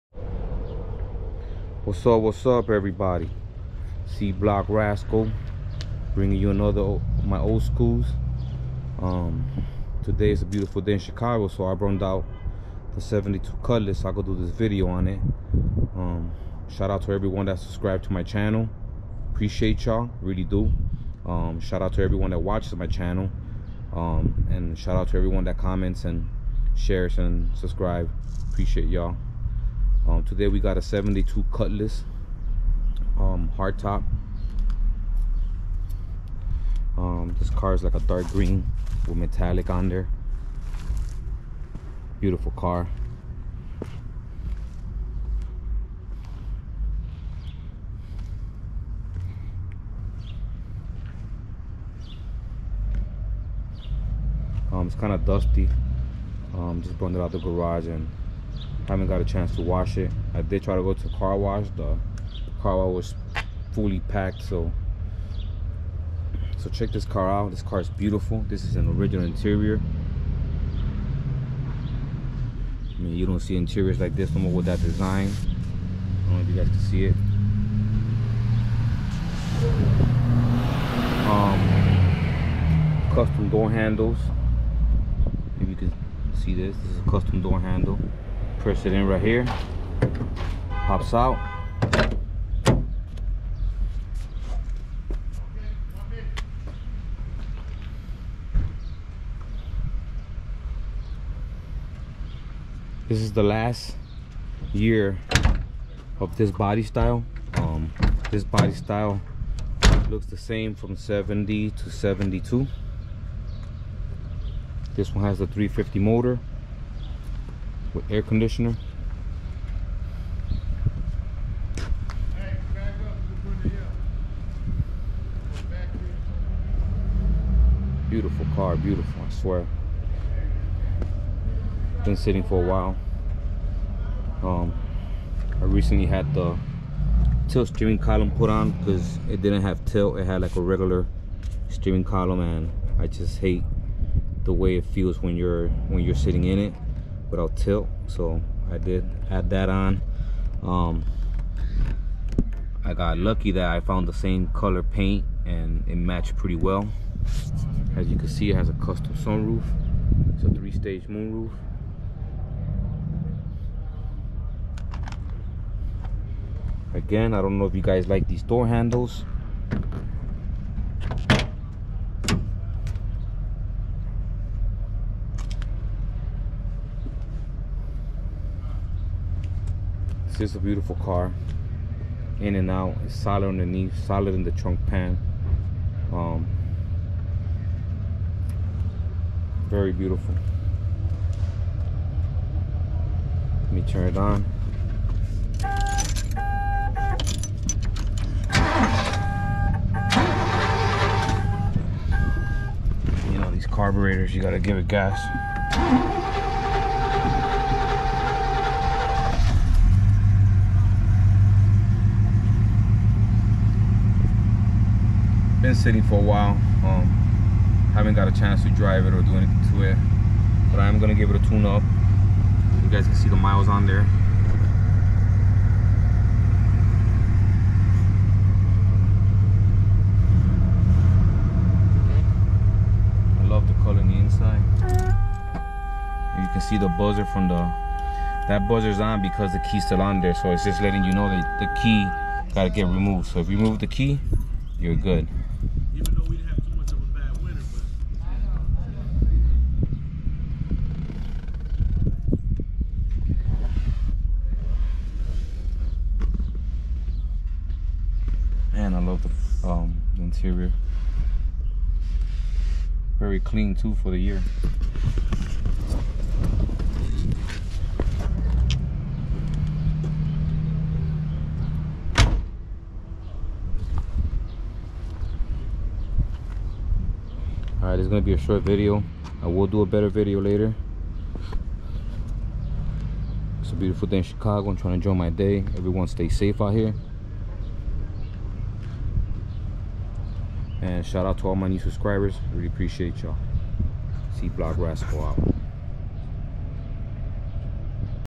1972 CUTLASS AKA GREEN MONEY 🤢 WITH THAT 3 STAGE ROOF FOR SALE
1972 cutlass  with that 3 stage moon roof will be for sale soon